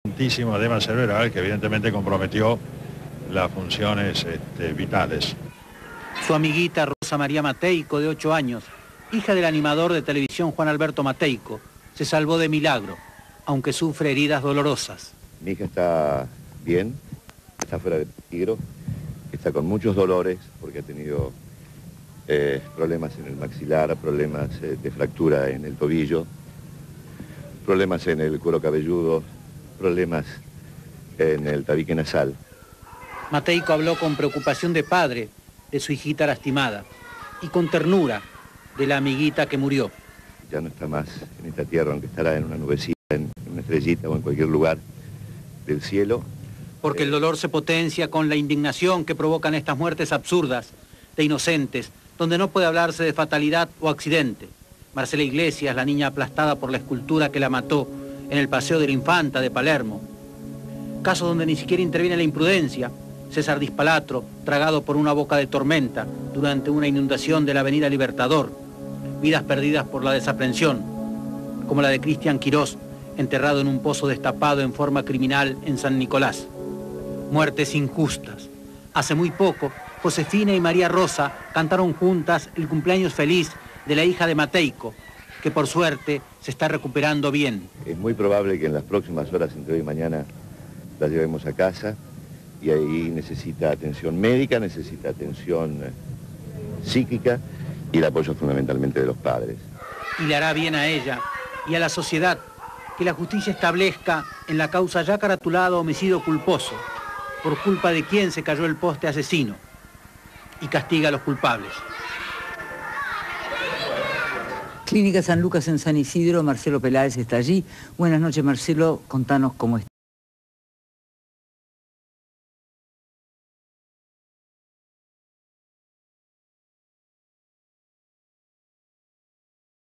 La tragedia de Juan Alberto Mateyko - DiFilm (1999)
Accidente  de su hija Rosa Maria y una amiga en un country Club los Olivos.  Declaraciones de Juan Alberto Mateyko. Parte medico.
Fecha:  5/10/99
Duración:
Código: V-00547